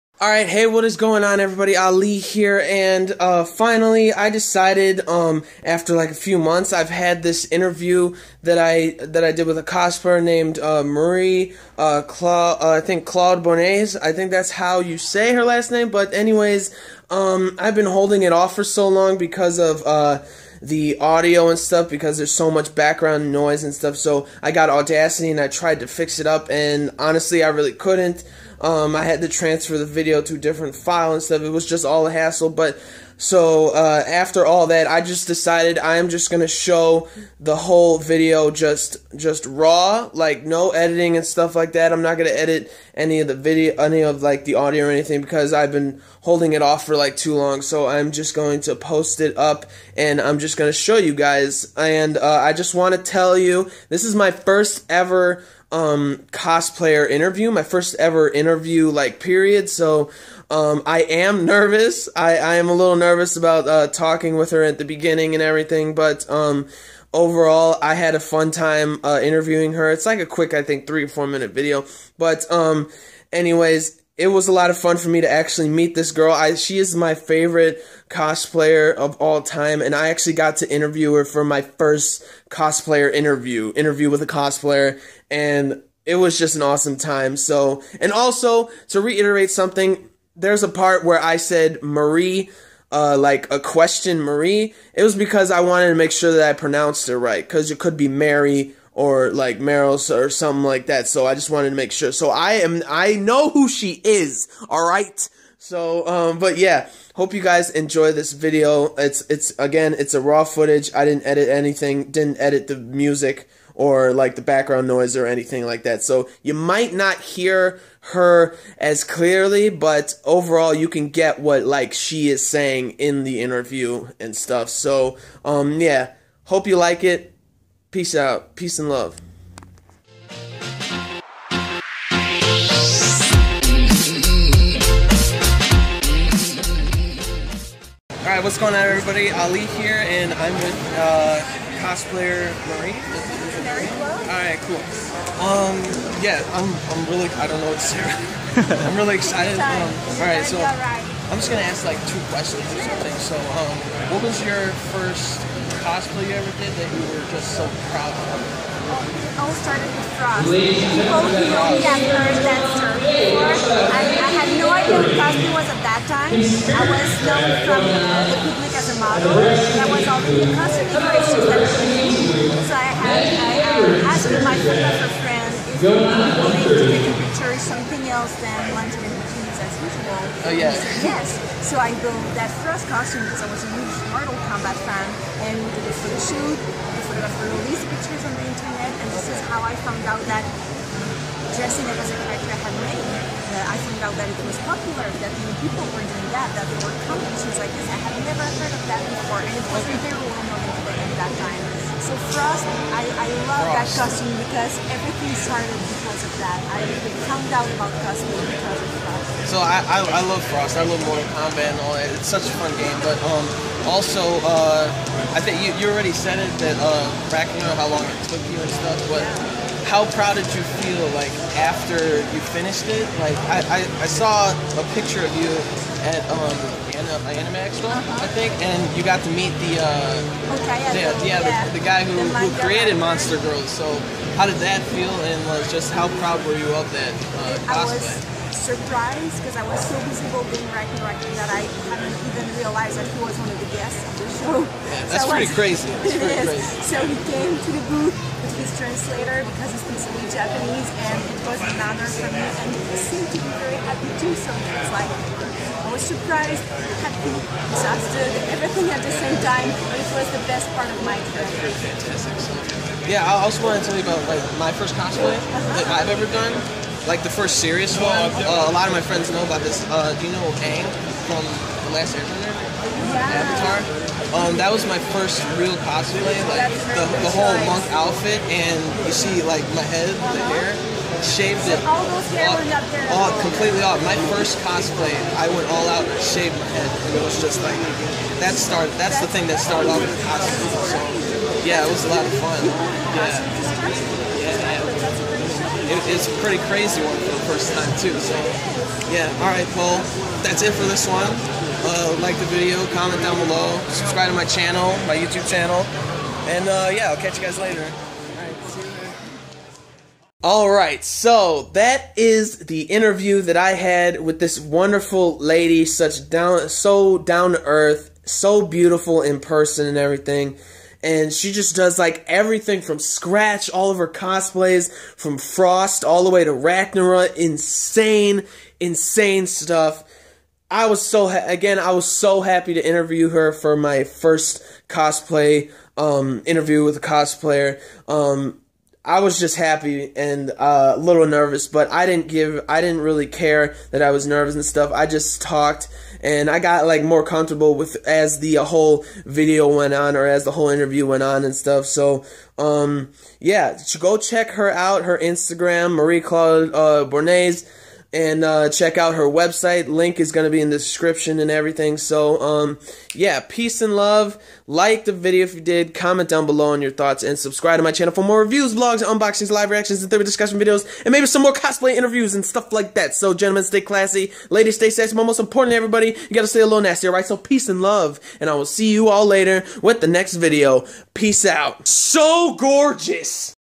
INTERVIEW WITH A COSPLAYER | Marie-Claude Bourbonnais!!!
I. LOVE. THIS. WOMAN. Such a cool, down-to-earth person, beautiful in person, a PHENOMENAL costume maker/cosplayer, and just such a comfortable person to be around. Again it was my very FIRST interview with ANYBODY, so it is a little rough :)